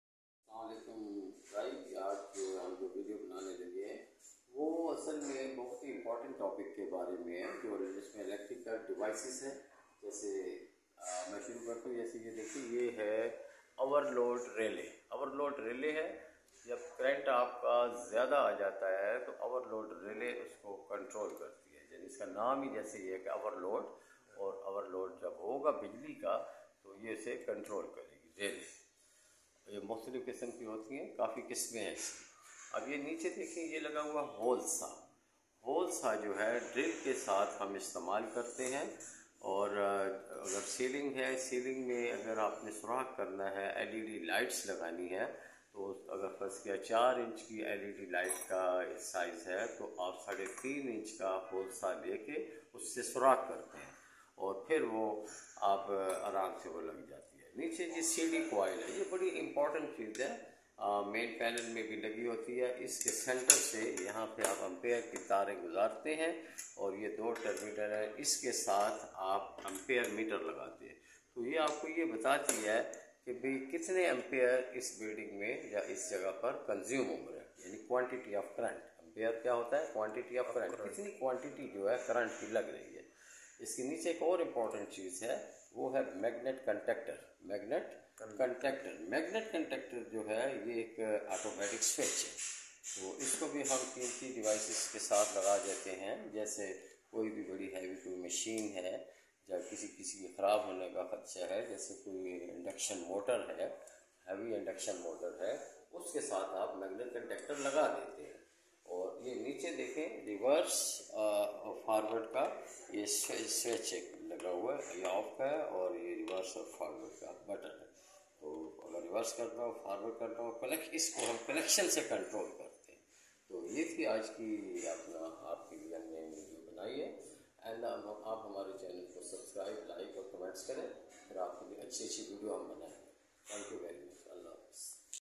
C T COIL, Overload Relay, Magnet Contactor, Whole Saw, use and presentation.
The best training  is offered in all Trades .